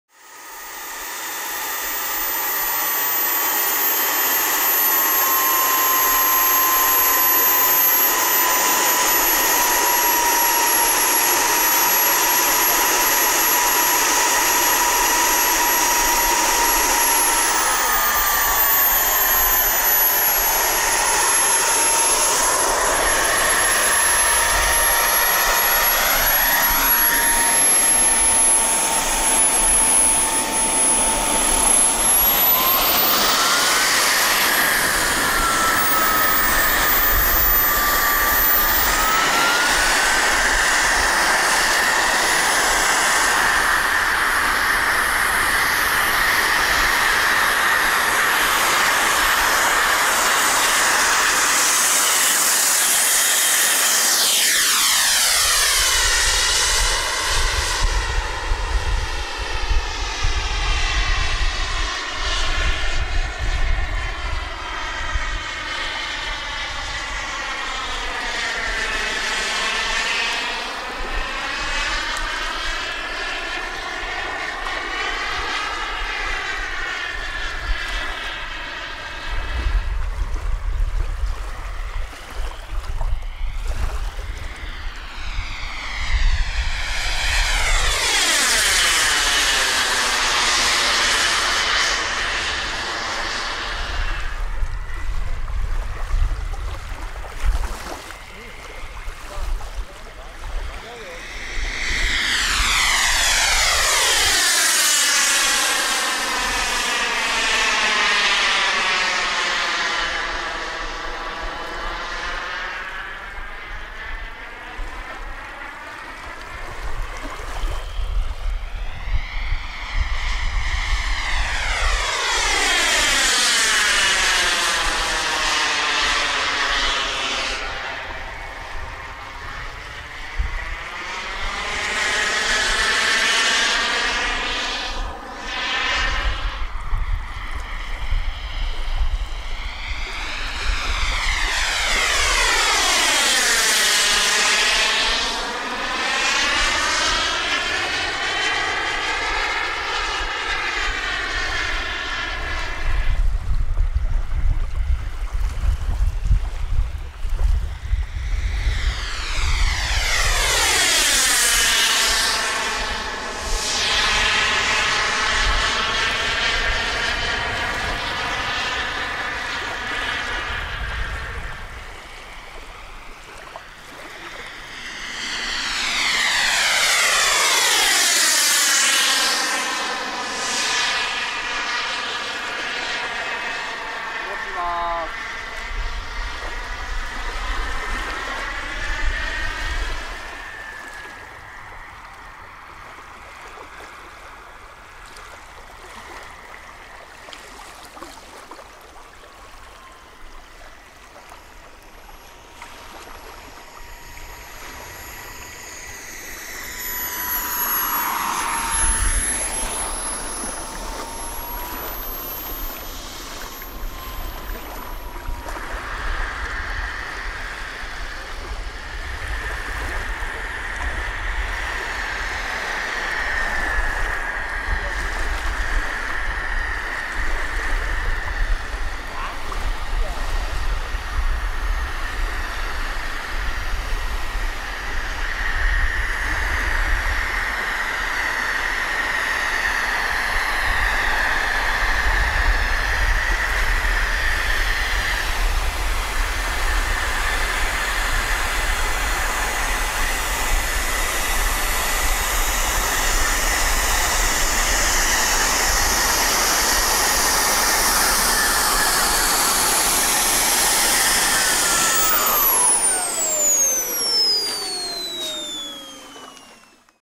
【HD】Jet Float Plane 'Yellow Marine' ジェットRC水上機
2020/2/24 霞ヶ浦にて撮影。製作と飛行は清水大輔氏。エンジンは
WREN 44。